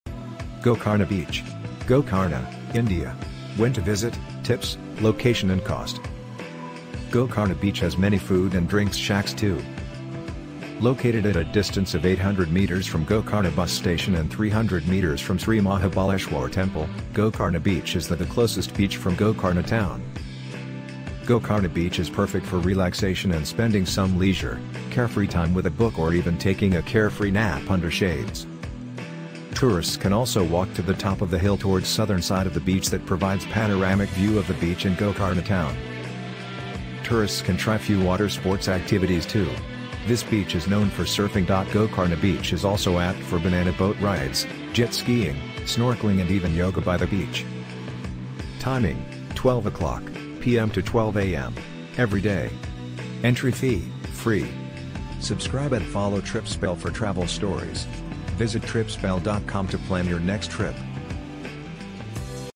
Gokarna Beach, Gokarna Guide - What to do, When to visit, How to reach, Cost Tripspell
Gokarna beach is perfect for some carefree time by the beach. Gokarna beach has many food and drinks shacks too. Do try surfing here and have some water fun.

Planning a Trip to Gokarna Beach in Gokarna, India?

Get all your travel questions answered about Gokarna Beach, Gokarna by visiting